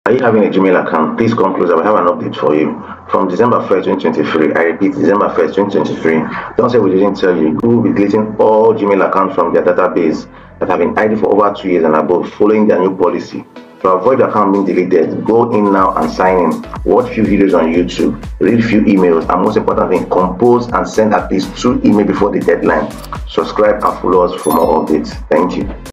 Avoid your gmail account to be deleted
This video will give you and insight on what to do. The new google policy will be doing clean up on their database for all idle Gmail accounts over two years and above.